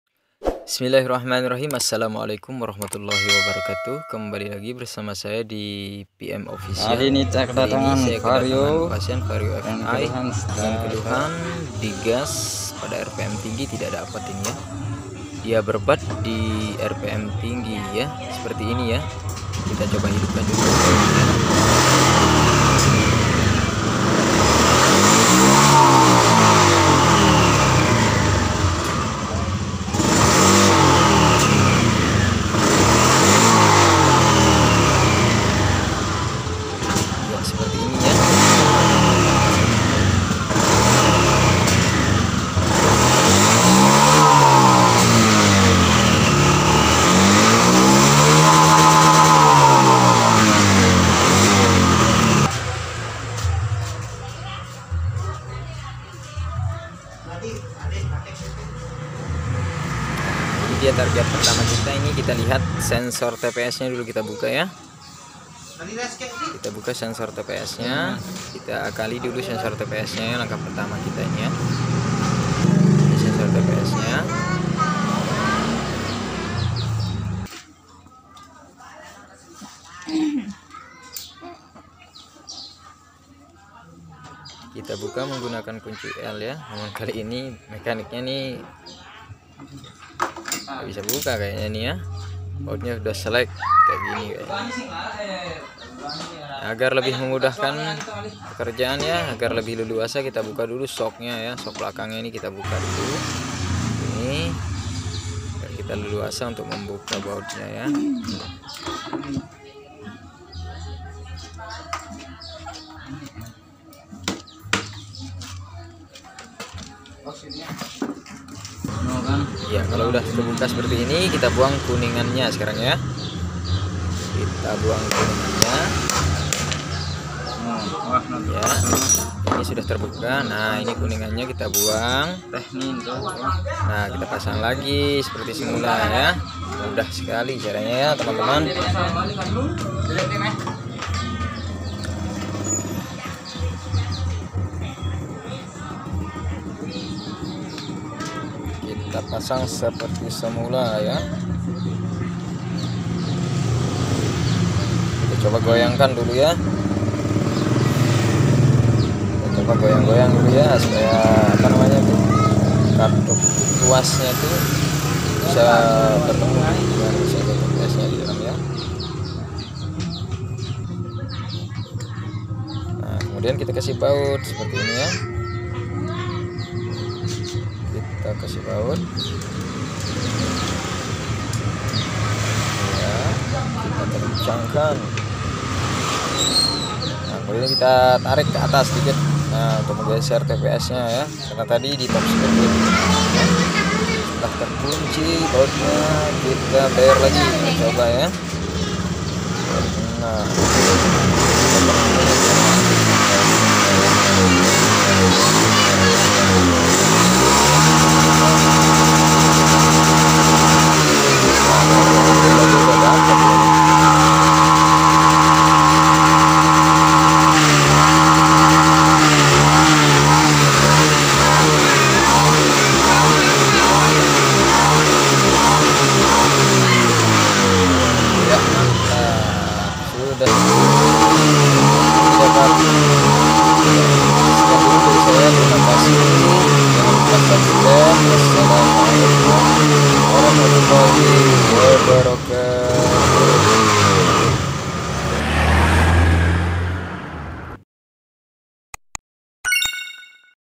Vario fi brebet di rpm atas|| buang bos Kuningan pada sensor TPS
honda Vario fi brebet diputaran atas,rpm tinggi seperti telat bensin

vario fi gas brebet
sensor TPS
geser tps